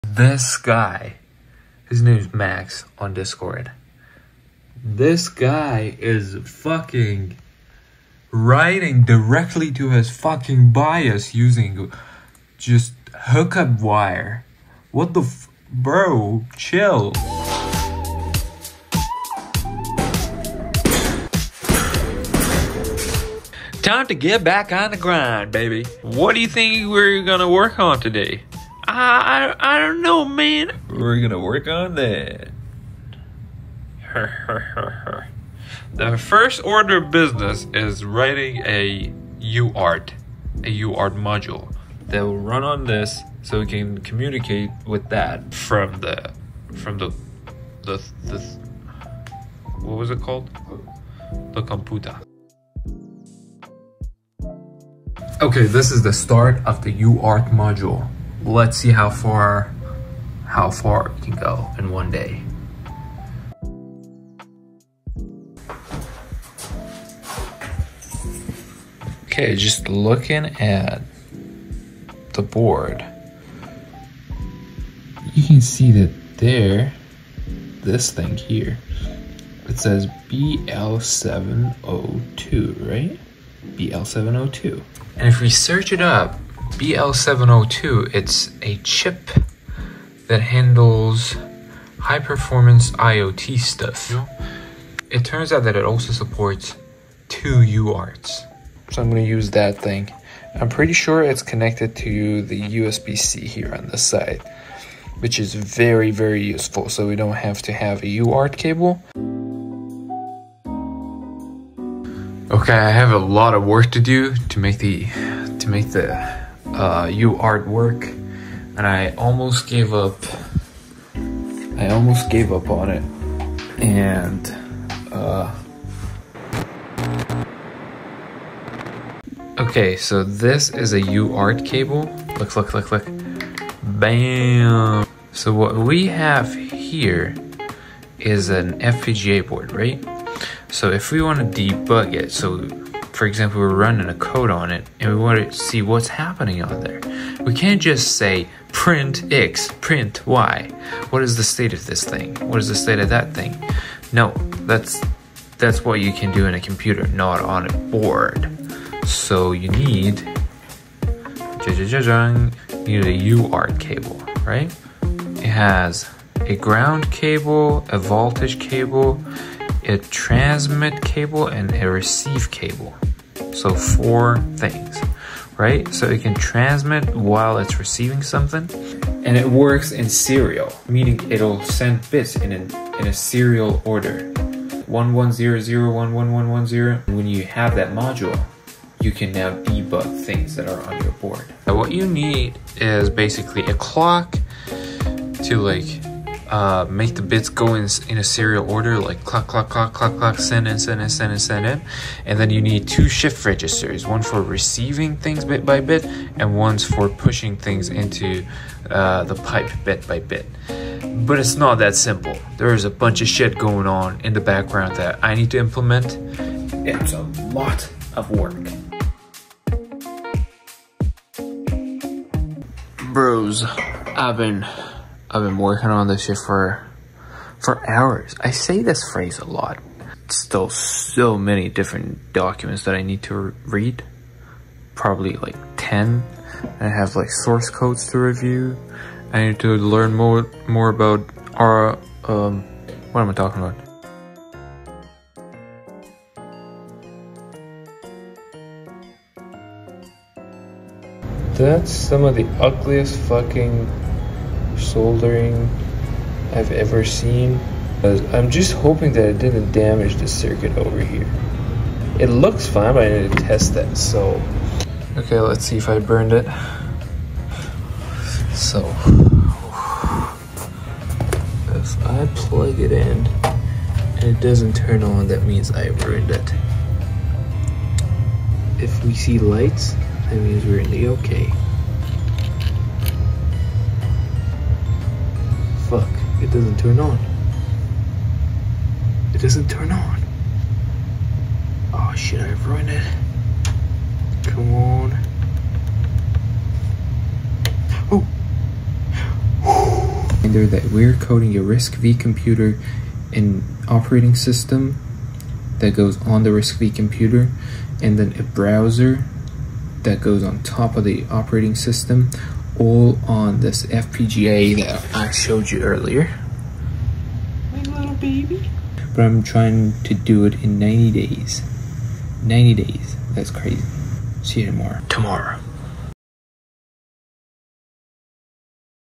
𝚂𝚝𝚊𝚛𝚝𝚆𝚘𝚛𝚔𝚒𝚗𝚐𝙾𝚗(𝚄𝙰𝚁𝚃); // 𝙳𝚊𝚢 𝟷𝟿
We’re getting serious about this computa thing

I started coding the uart module. We’re officially on our path towards our own custom computer!

Github.com/willlogs
Willsamadi.com